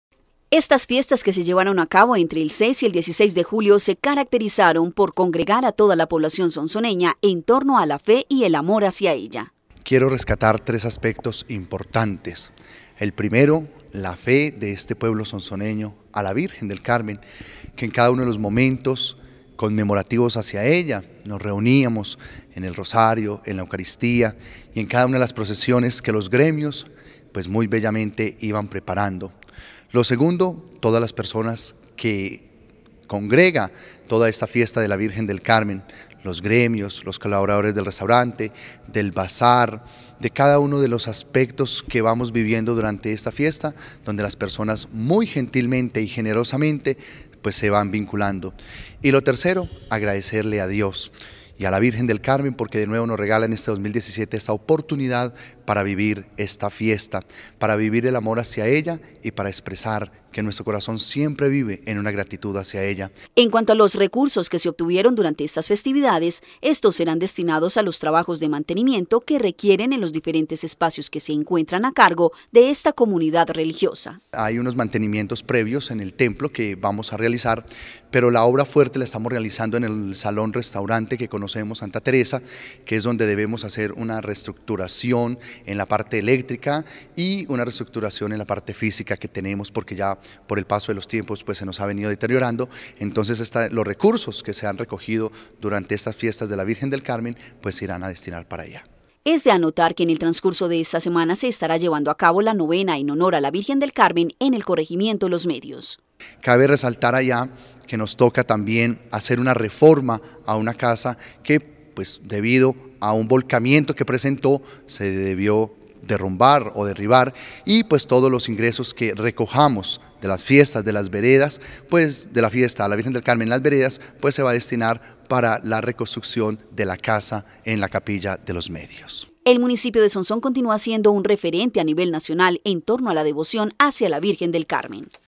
FIESTAS EN HONOR A LA VIRGEN DEL CARMEN EN SONSÓN
La comunidad de padres carmelitas entrega un balance satisfactorio con relación a las Fiestas patronales en honor a la Virgen María, bajo la advocación del monte carmelo.